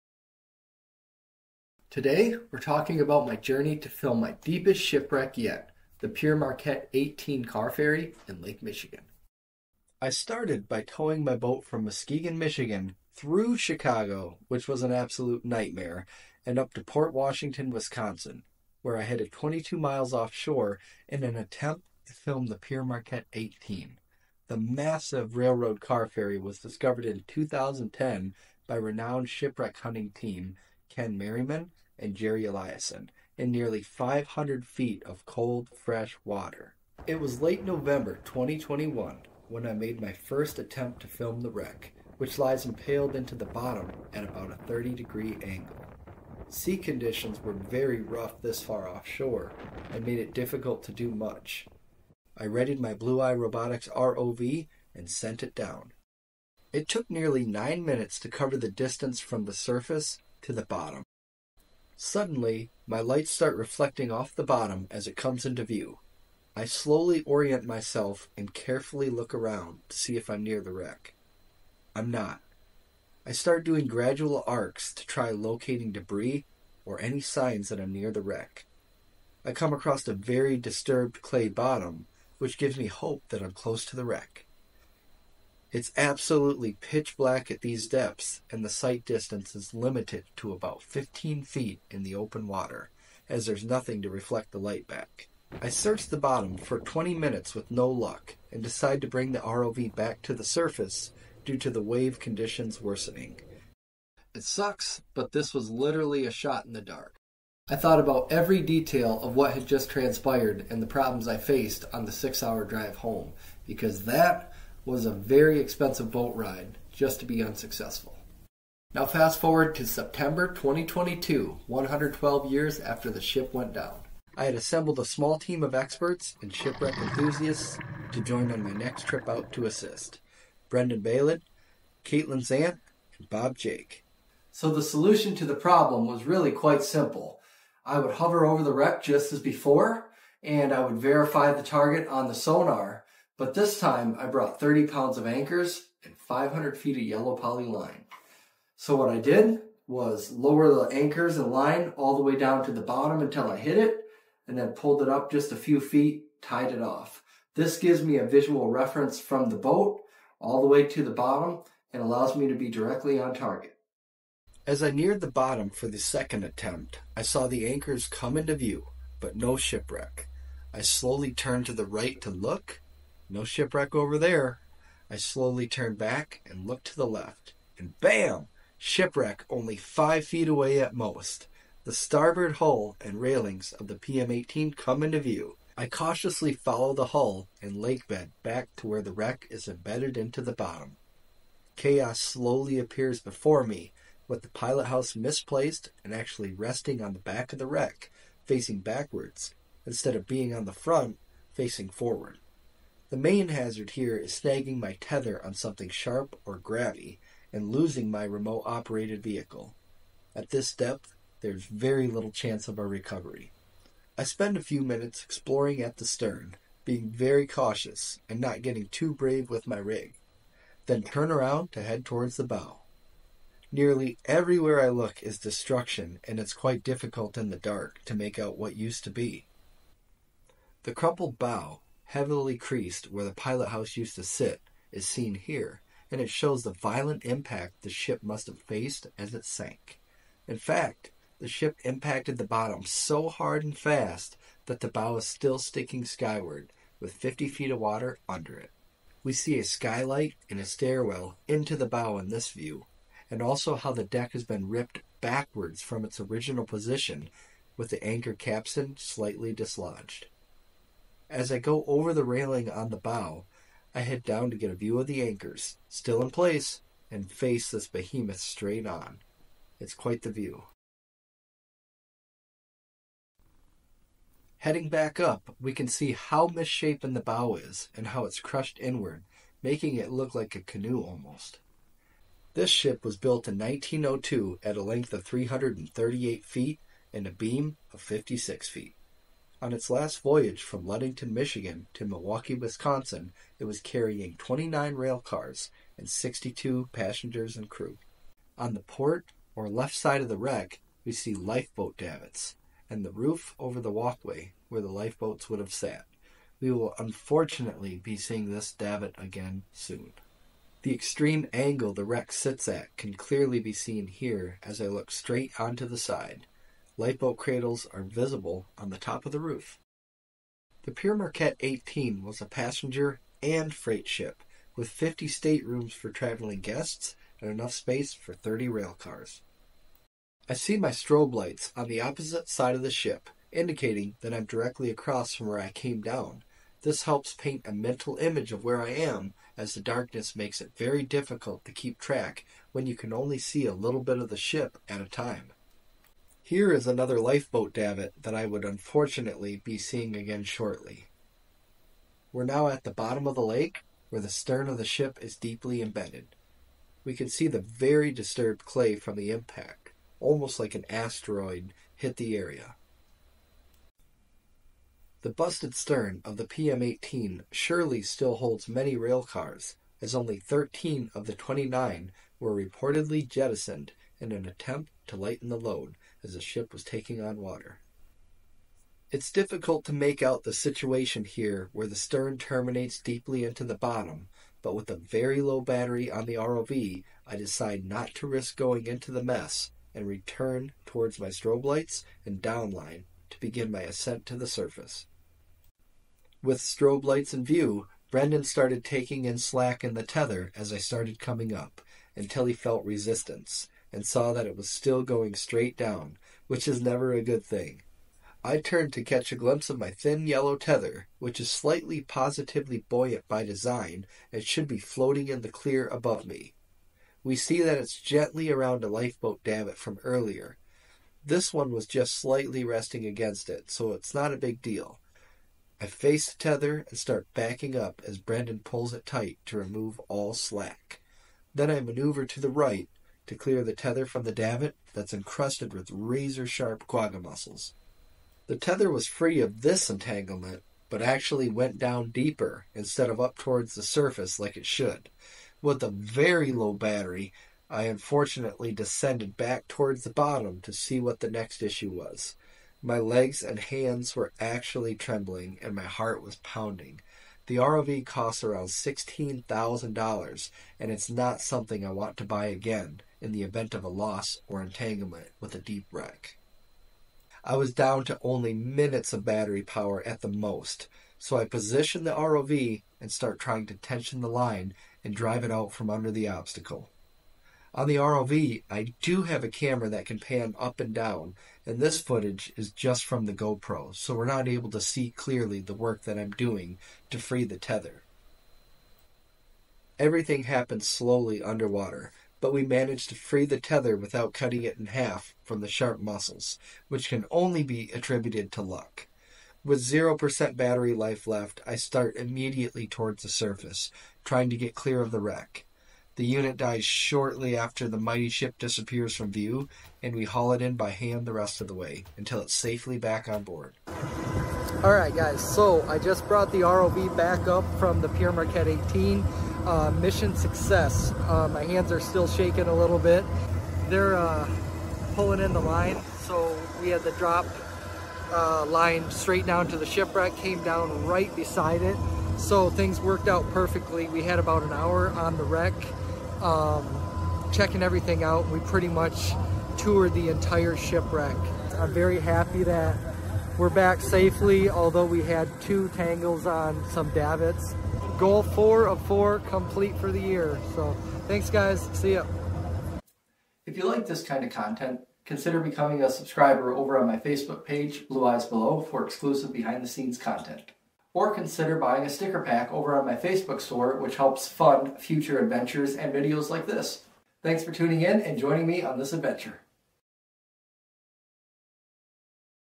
Pere Marquette 18 ROV Exploration
Discovered nearly 500 feet deep in Lake Michigan, the Pere Marquette 18 sits at about a 30 degree angle, speared into the lake bed from sinking quickly. Join me as I show the process to document this historic shipwreck.